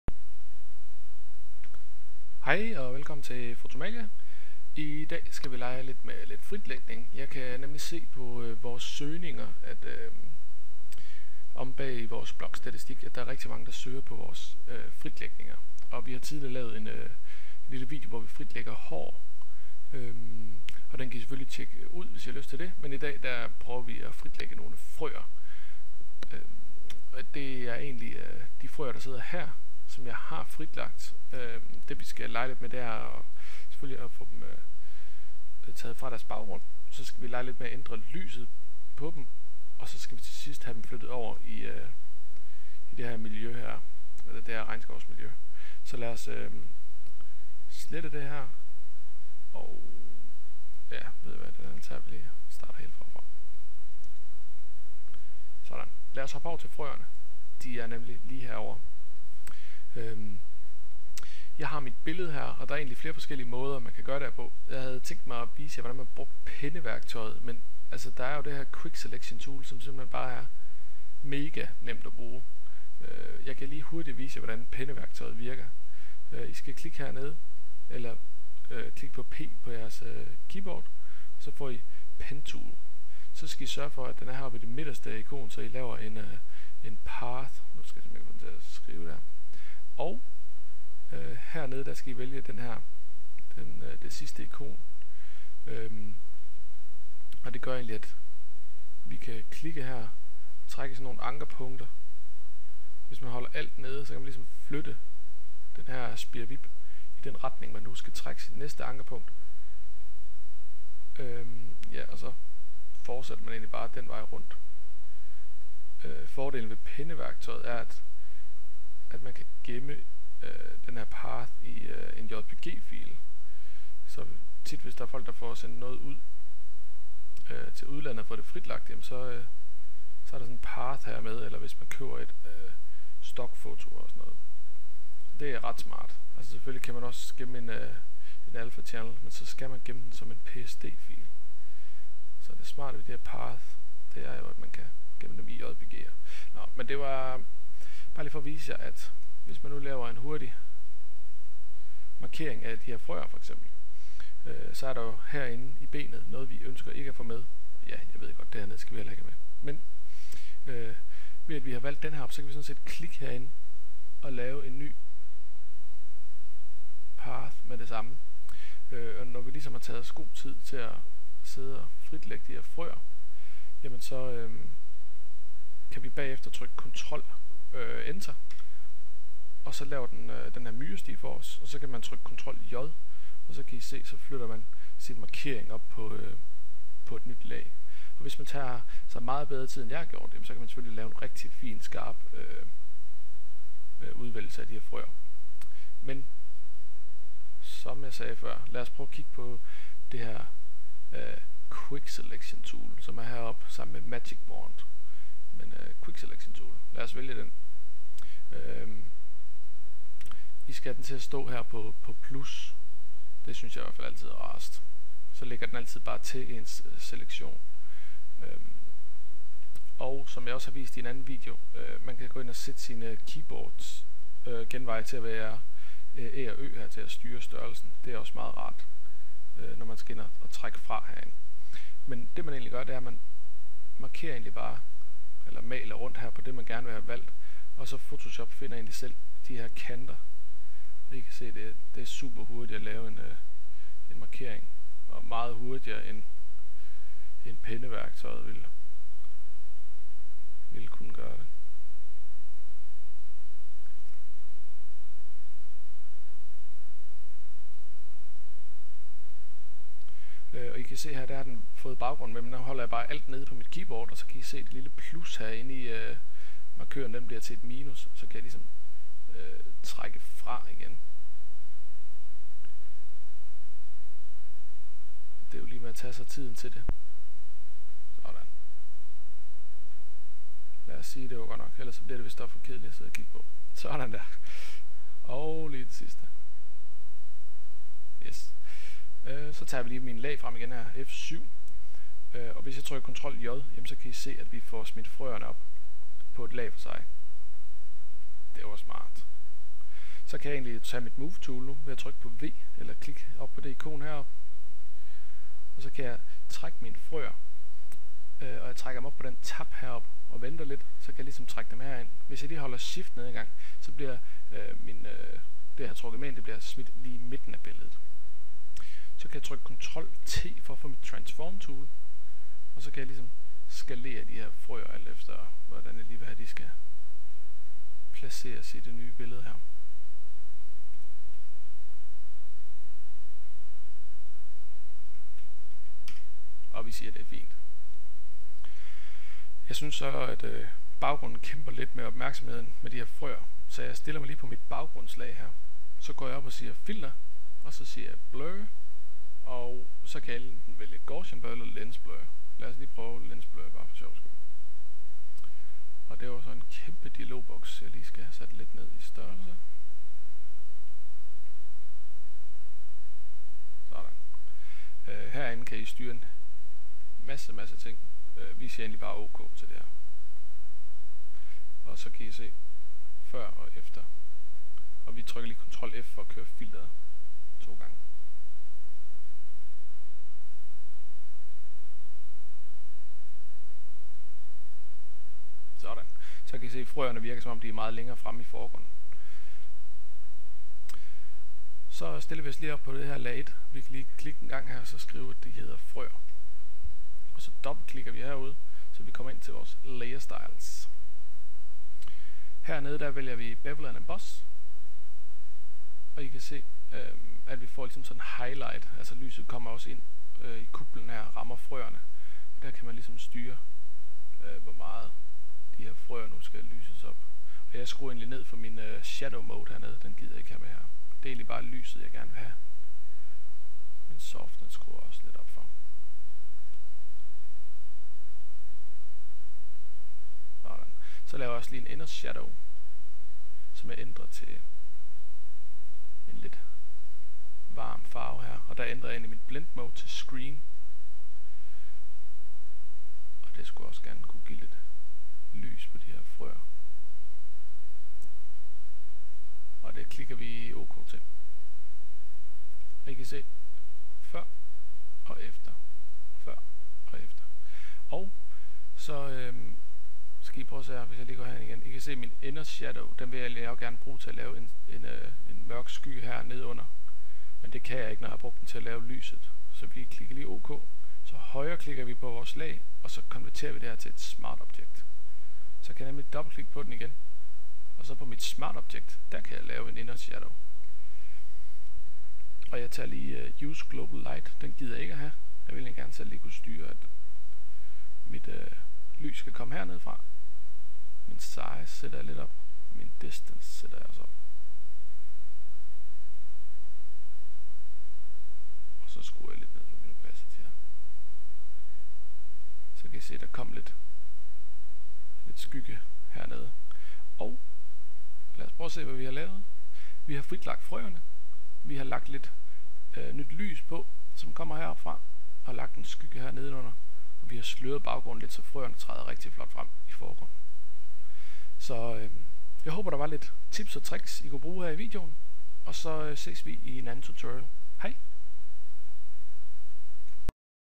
fotomalia.dk/blog - Fritlægning
I denne video viser vi, hvordan man kan fritlægge et objekt i photoshop.